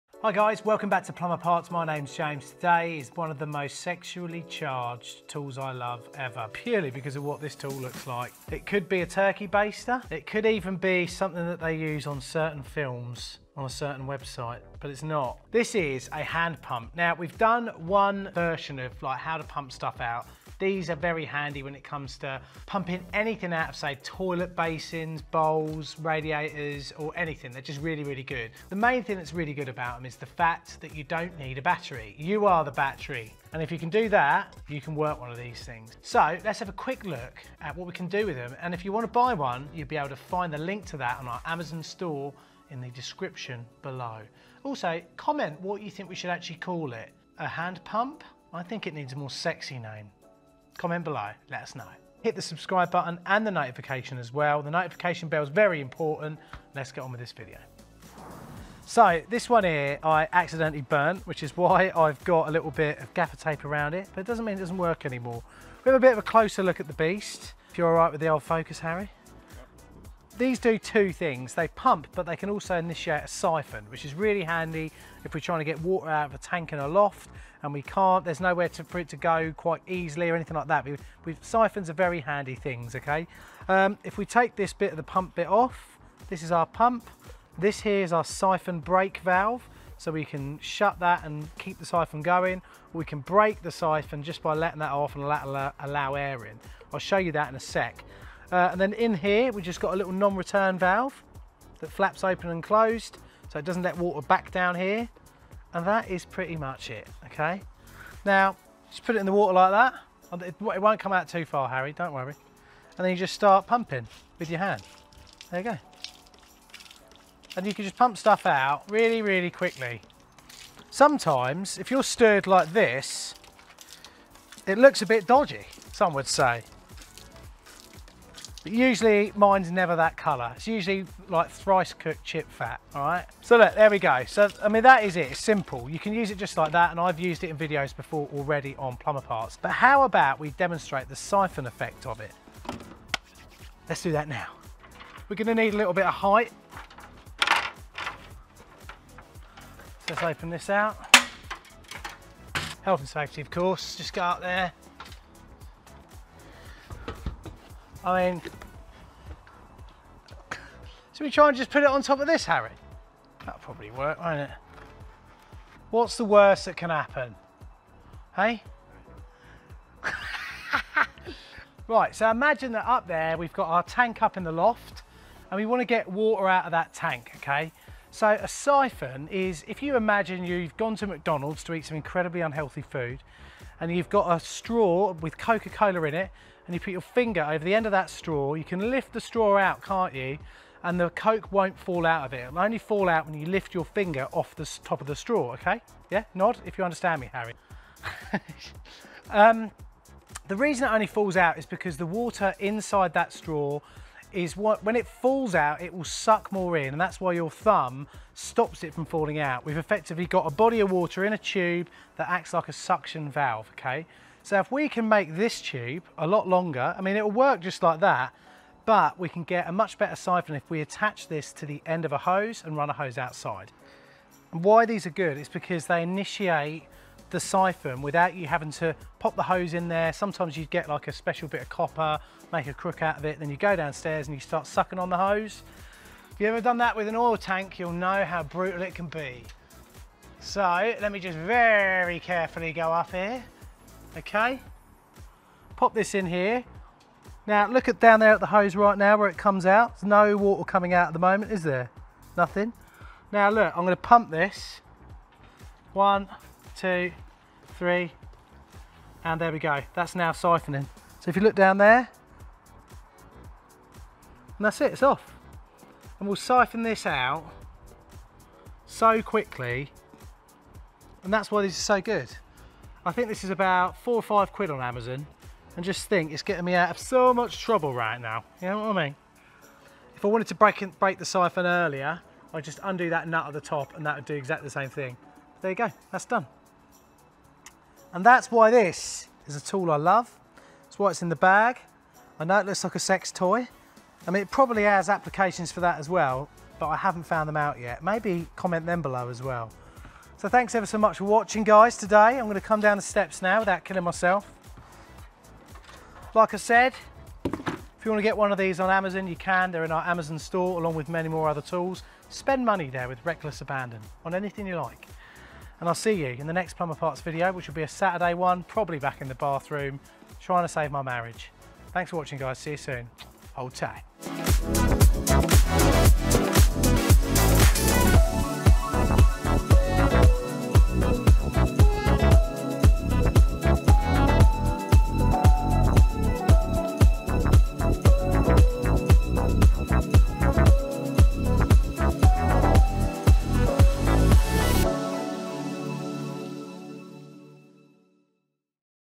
Hand Pump Water - Must have tools of the trade
Ever wanted to hand pump water? Always annoyed about getting the wet vac from the van and lugging it upstairs to that toilet cistern? Easily start a siphon? MUST HAVE!

If you can't have a motor running, or plug in a wet vac then a hand pump is the perfect tool to start a siphon or clear out a toilet bowl, toilet cistern or anything else you want to empty. This costs about £7 and is wicked. This beast hand pump clips to my tool bag. It's a tool I love for the plumbing trade! It's a plumbing tools review! Connects to your hose easily to drain any tank or radiator. It's definately one of those plumbing tools you need.